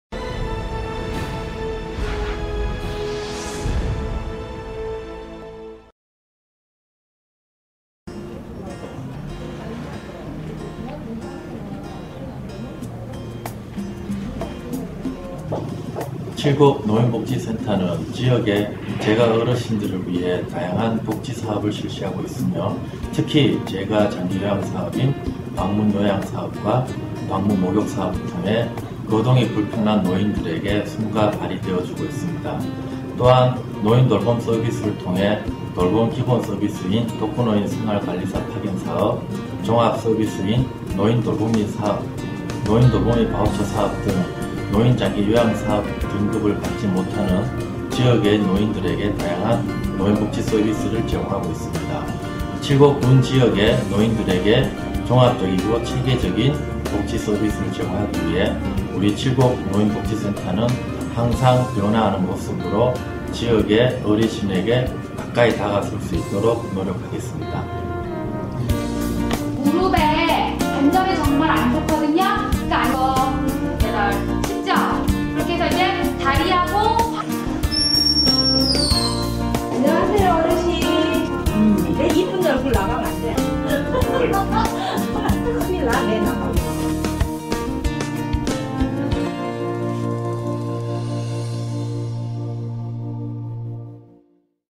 칠곡노인복지센터 (Chilgok Home Helper Dispatch Center)
[한국향토문화전자대전] 칠곡노인복지센터 (Chilgok Home Helper Dispatch Center)
2006년 칠곡군으로부터 재가노인복지시설로 승인 받은 뒤 칠곡군에 거주하고 있는 저소득 노인들에게 가정봉사원 파견사업, 결연후원사업, 생활지원사업, 문화지원사업 등 다양한 사회복지서비스 제공을 주요사업으로 하고 있다.